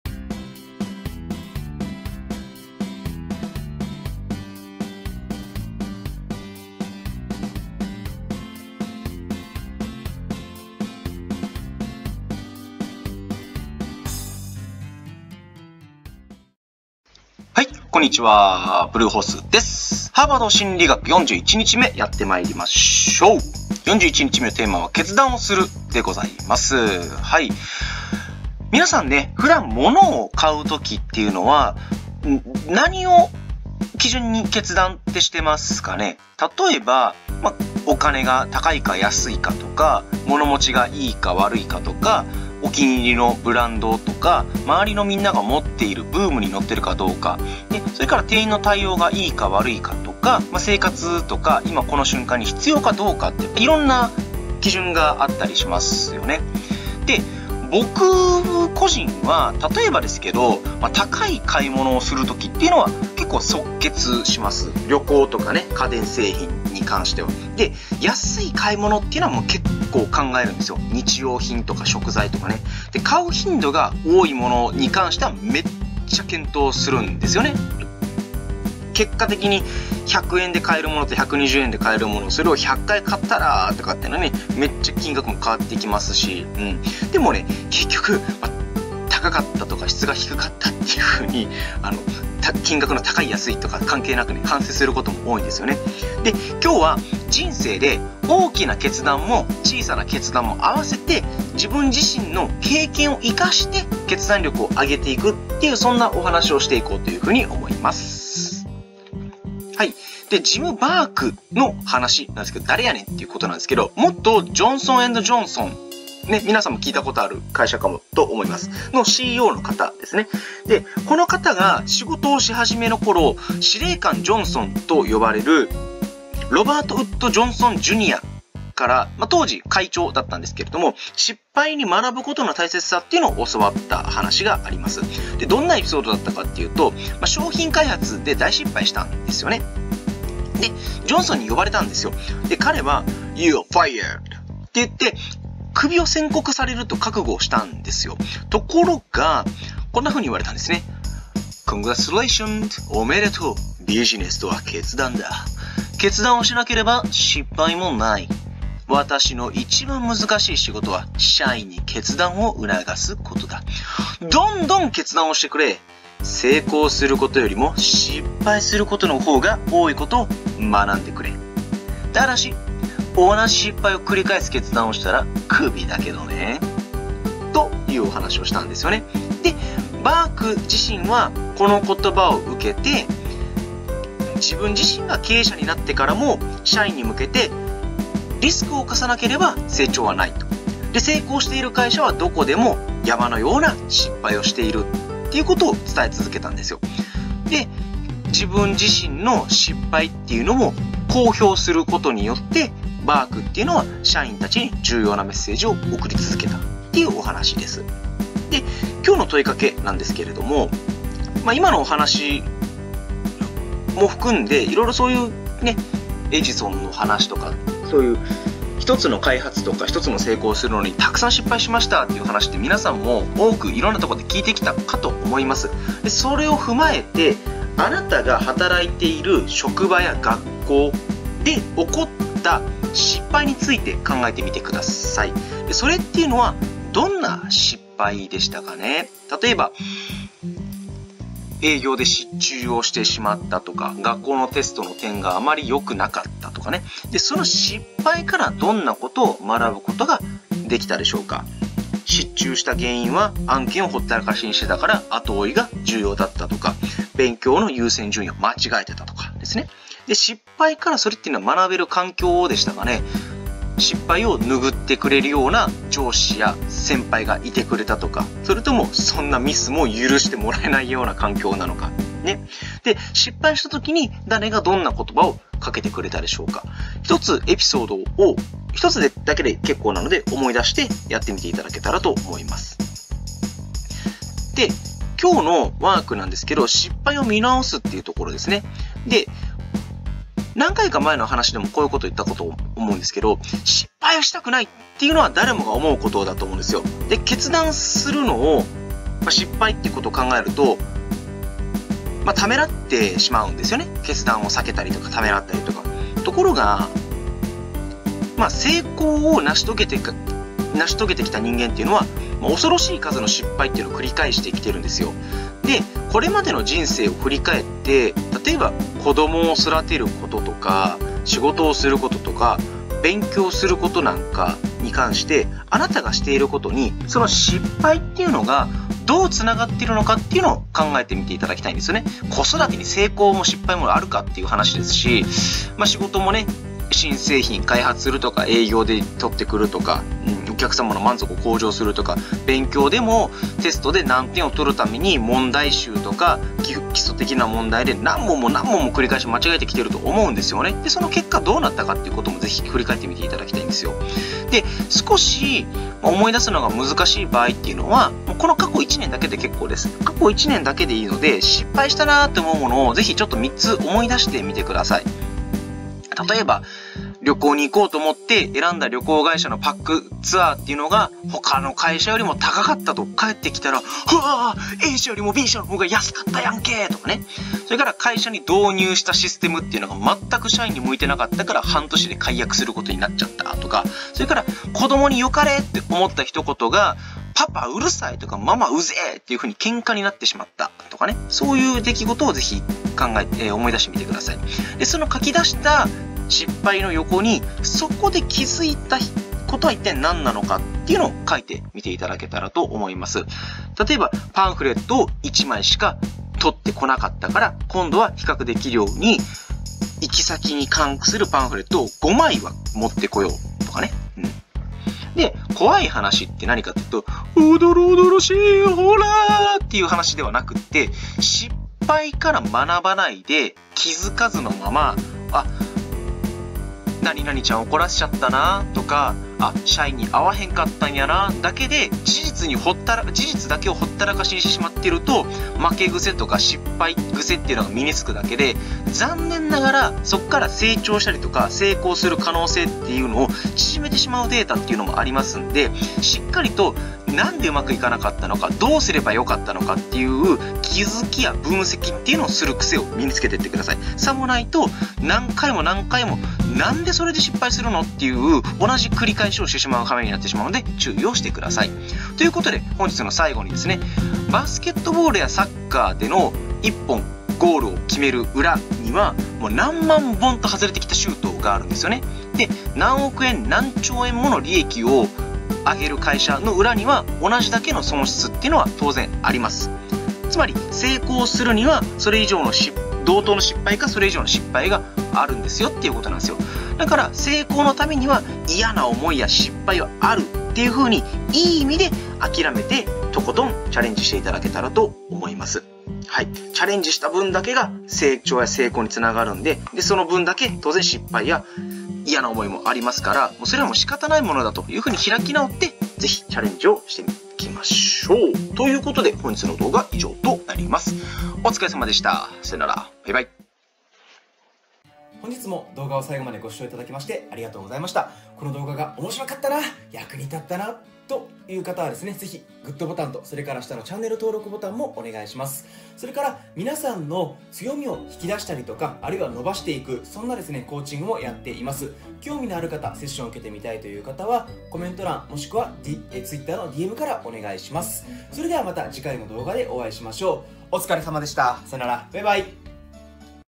【ハーバード心理学】41日目：決断をする
高額なものは即決！の決断力！が・・・だいぶ反省材料になりますね・・・
的確な決断力を身に着けるための話っす！